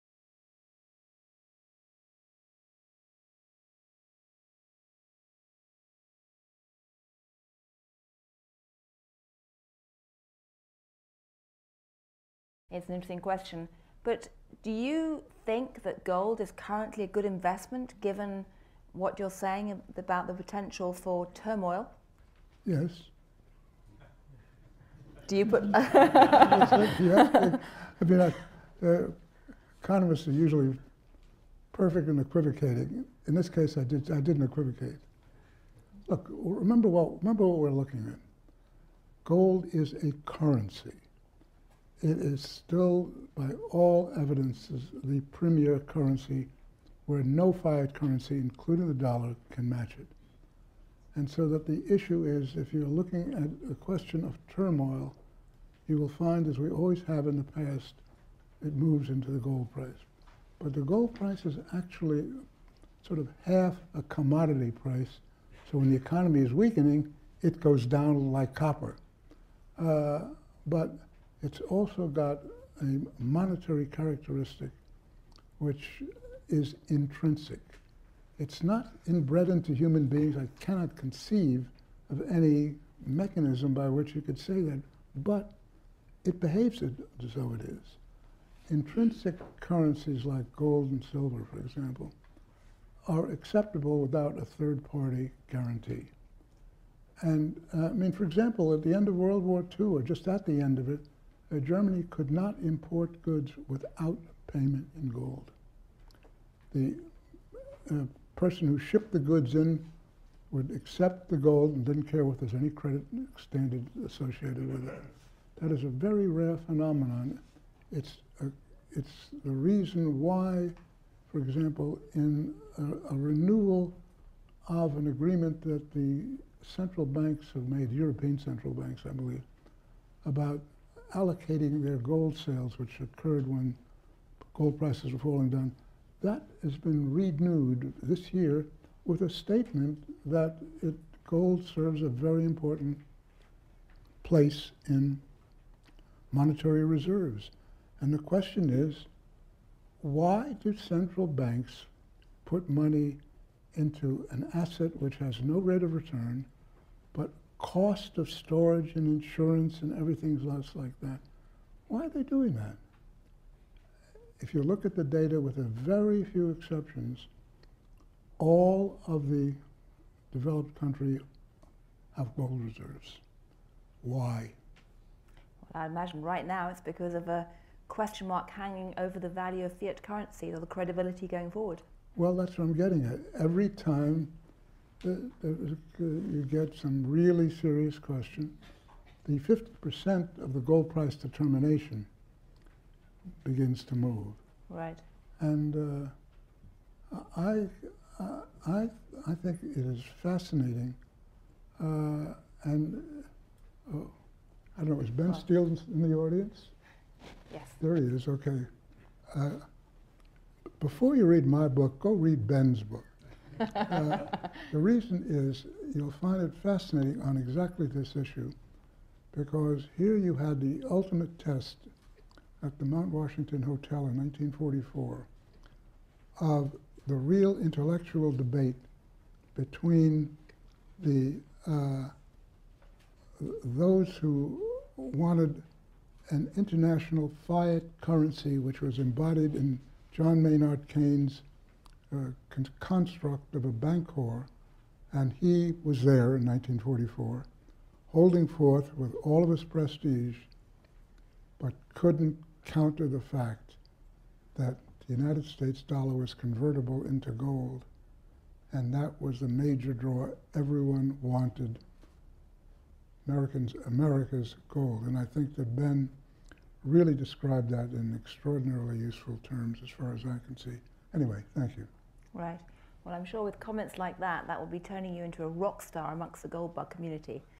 Alan Greenspan Oct 2014: Gold Is THE Premier Currency
Alan explains there is only one real currency: gold.

For some reason, the Council of Foreign Relations, where ex-Fed-Chief Alan Greenspan spoke, decided the following discussion should be left out of the official transcript. We can perhaps understand why... as Gillian Tett concludes, "comments like that will be turning you into a rock star amongst the gold bug community."